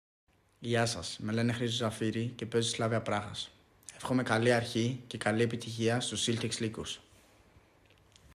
Ο Χρήστος Ζαφείρης για τους ΥΛΤΕΞ ΛΥΚΟΥΣ 🐺
💥Η ακριβότερη πώληση στην ιστορία της Χάουγκεσουντ, το νούμερο 🔟 της Σλάβια Πράγας, ο δικός μας Χρήστος Ζαφείρης

🙏🤝Χρήστο, σε ευχαριστούμε για τα καλά σου λόγια